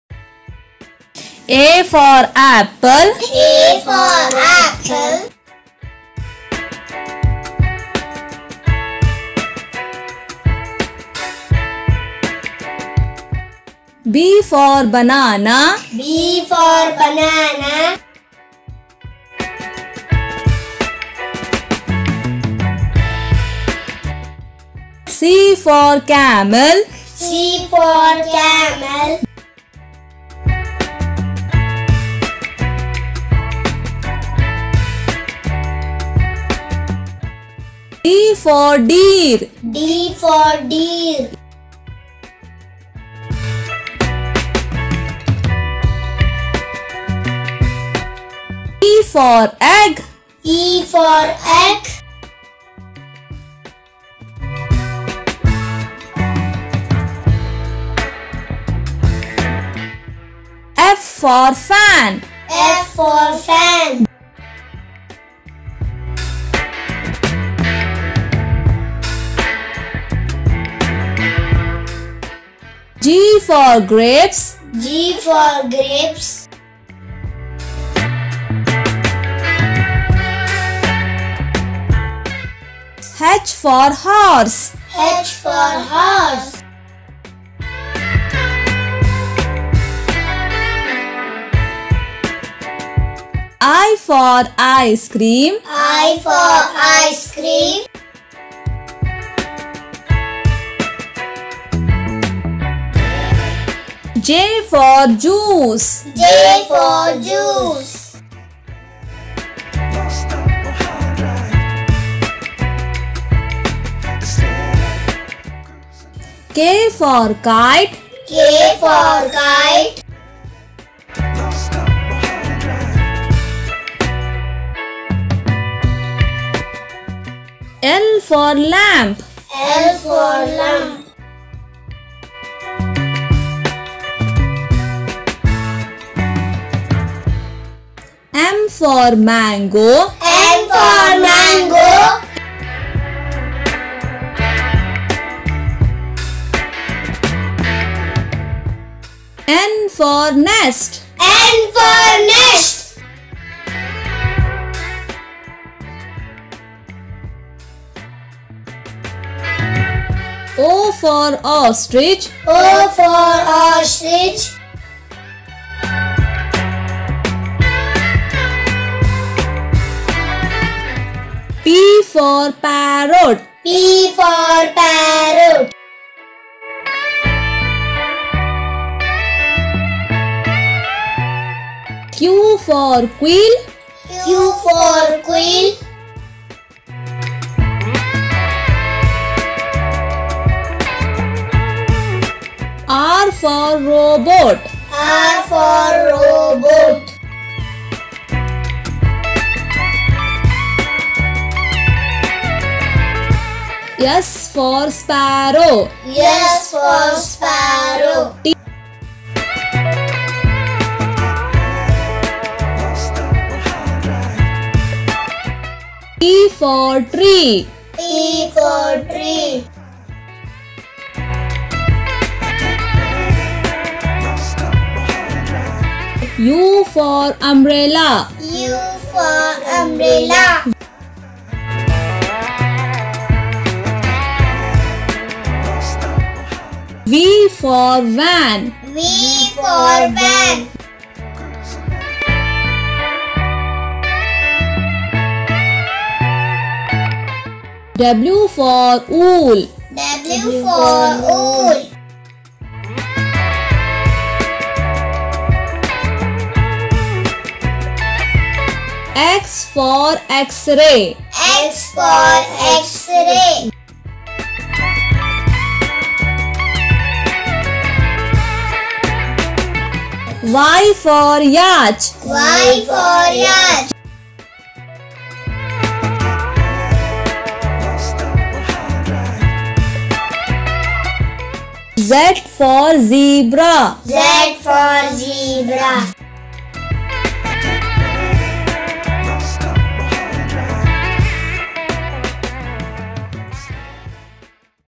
a for apple abcd | English alphabet | how to write ABCD | Kids Class chote bachhon ki padhaai
c for camel
d for deer
e for gg
q for quill
r for robot
s for sparrow